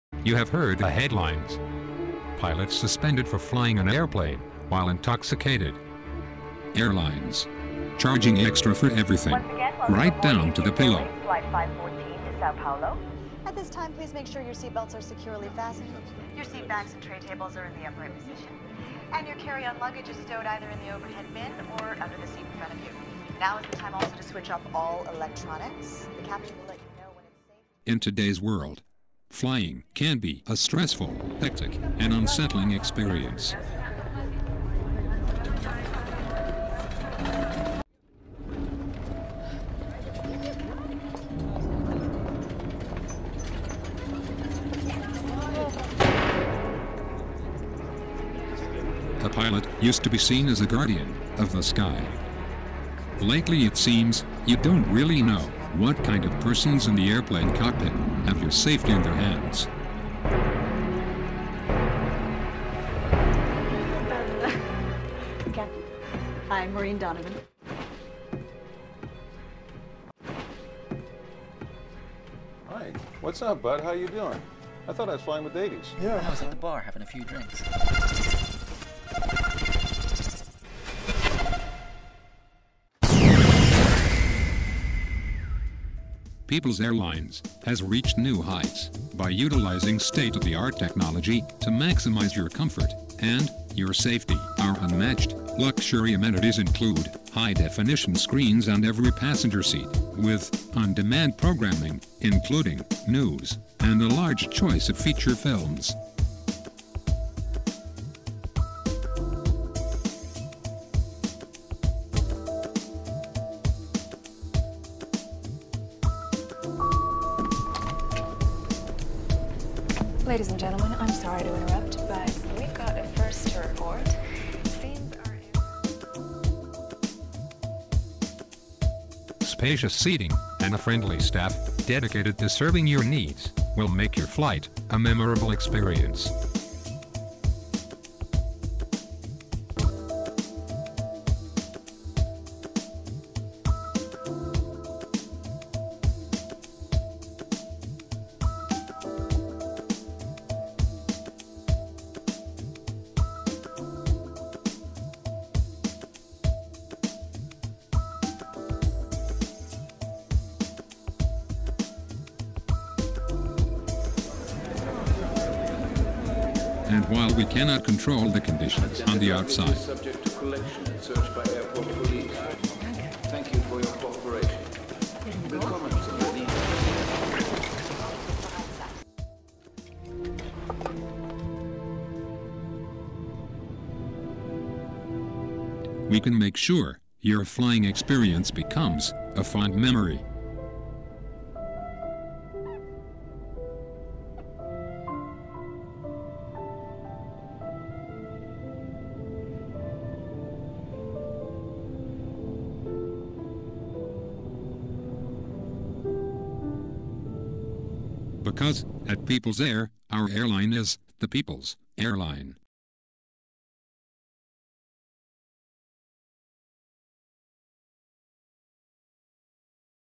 commercial Airlines and their Pilots: can they be trusted with your Life
You've heard the headlines. Pilots flying airplanes while intoxicated or while exhausted from being overworked.

The state of economics in the commercial airline business of today is such that it forces Airlines to cut corners on quality of service. So they overwork their pilots and charge you for everything.

Under such conditions, how comfortable can you really feel about flying a commercial airline?

This Public Service Announcement ( PSA) style video ad addresses these concerns.  A similar video commercial could be produced for an airline or aviation company of any size.